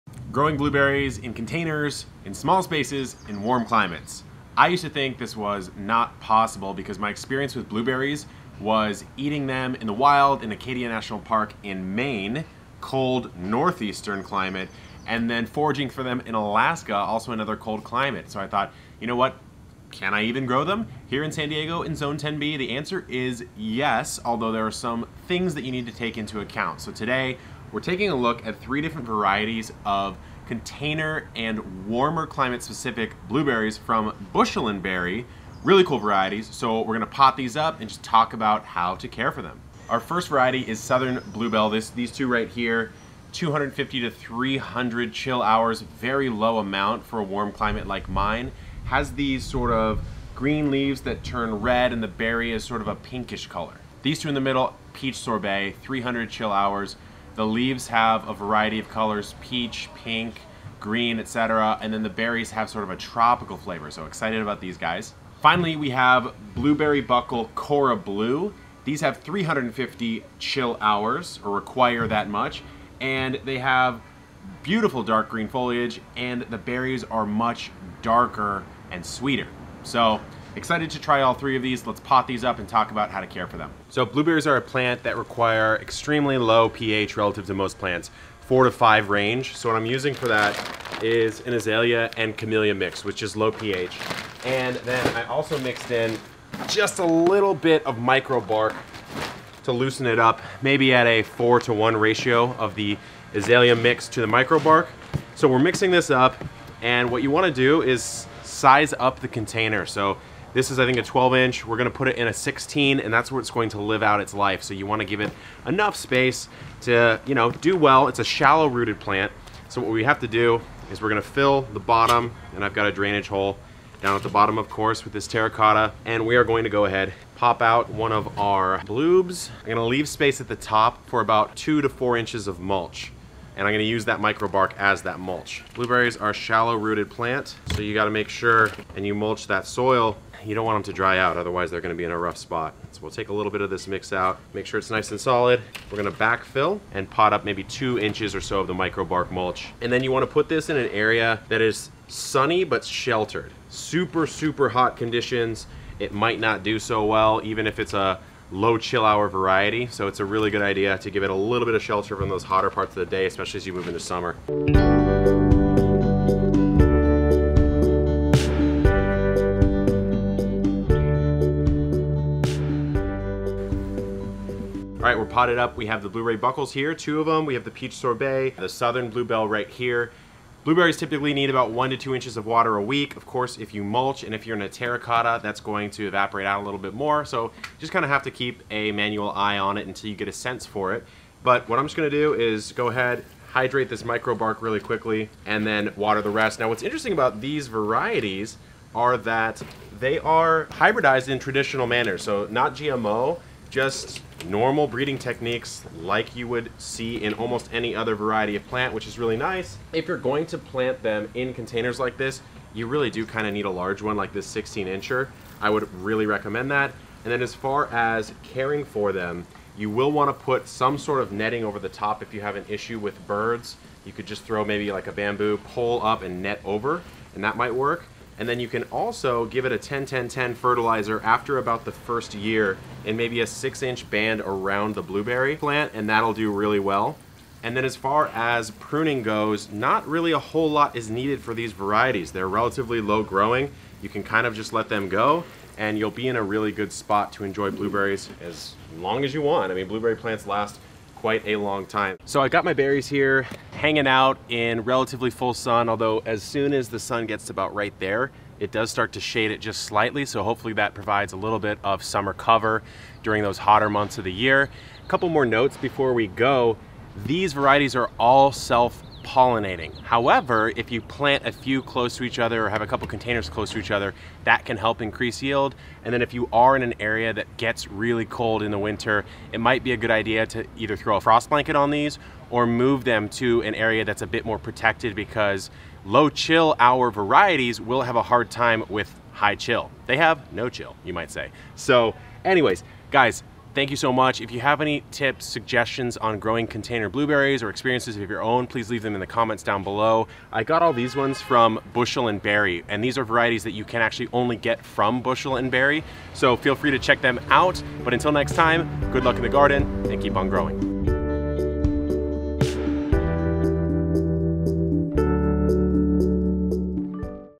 How to Grow Blueberries in Containers: Soil and Planting
If you live in a warmer climate but still want to grow delicious blueberries...you're in luck. I actually thought this wasn't possible, but got my hands on some low chill hour varieties that work really well for smaller spaces.

So if you want to grow blueberries in containers, these are some of my tips for getting them planted, making sure your soil pH is correct, and mulching so you don't lose moisture as they're quite a shallow-rooted plant.